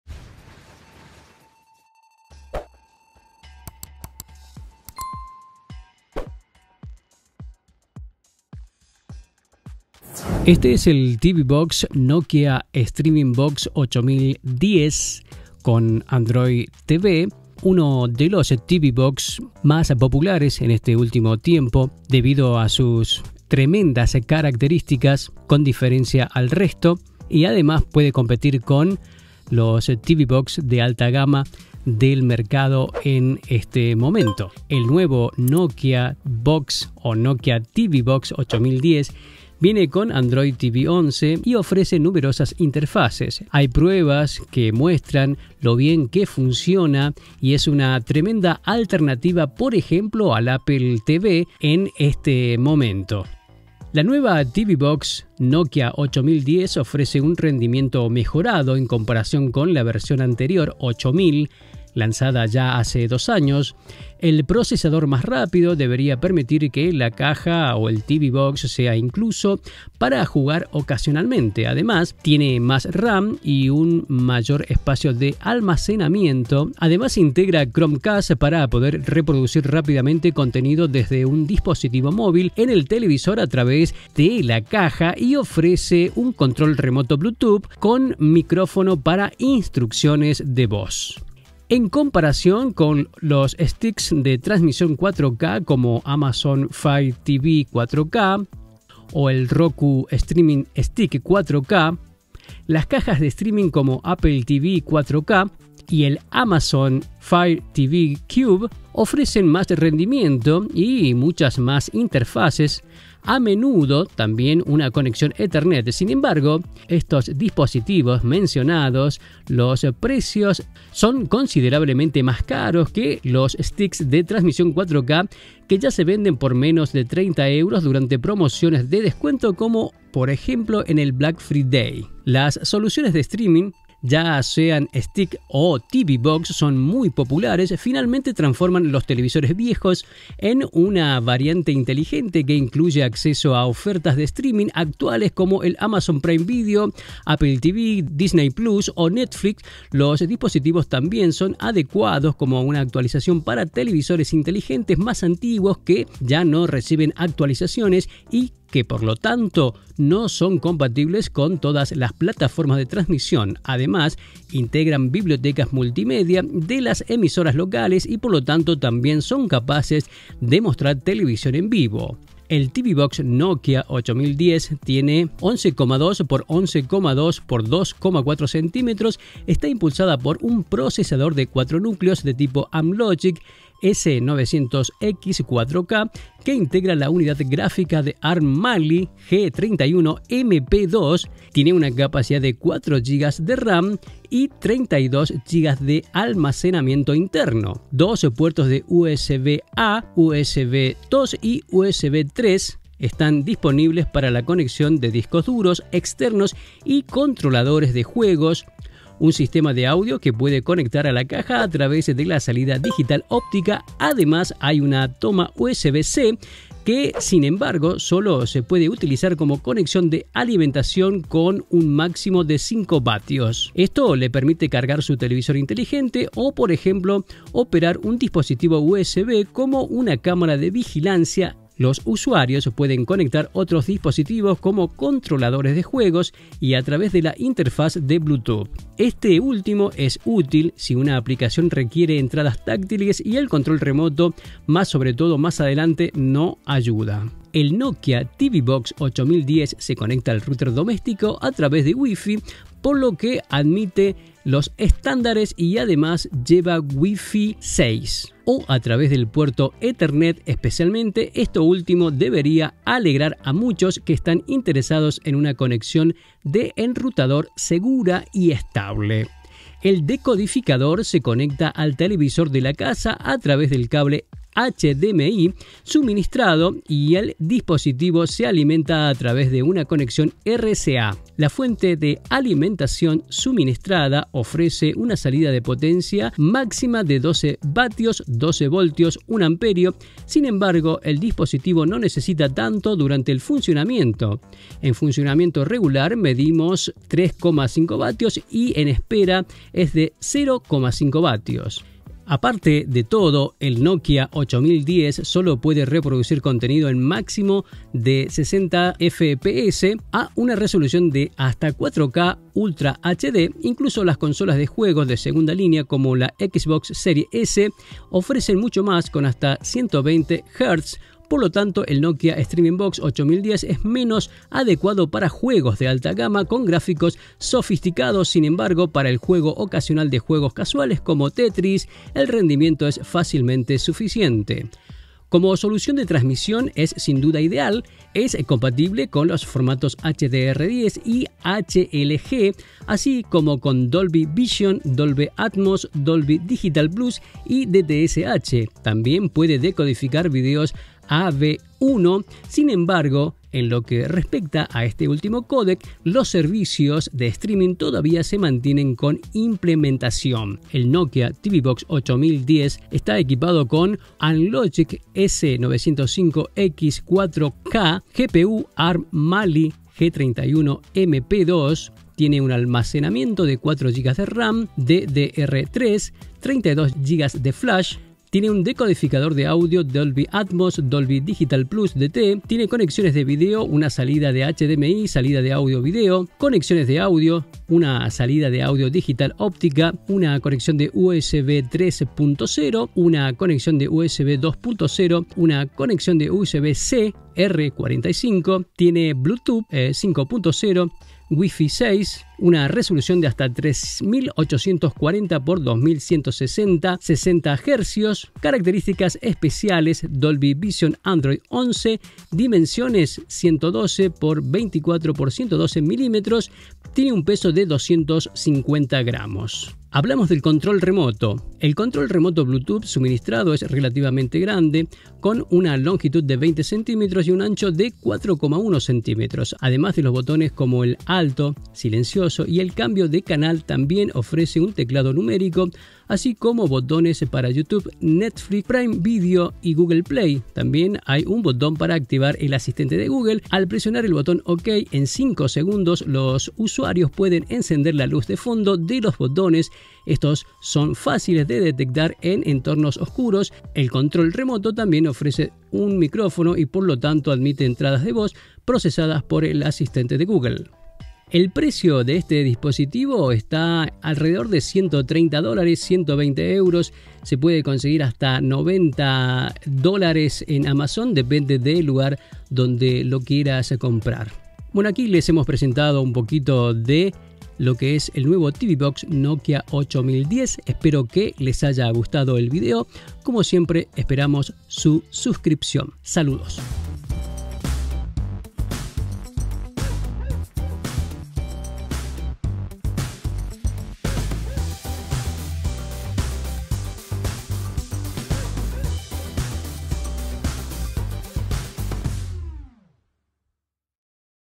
NOKIA Streaming TV Box 8010 •en español•
Nokia Streaming Box 8010: para Netflix, Amazon Prime, Disney+, YouTube y más. Android 4K Ultra HD Box con funciones de streaming y reproductor multimedia.

Convierte cualquier televisor en un televisor inteligente con Android.

                    ♥︎GRACIAS♥︎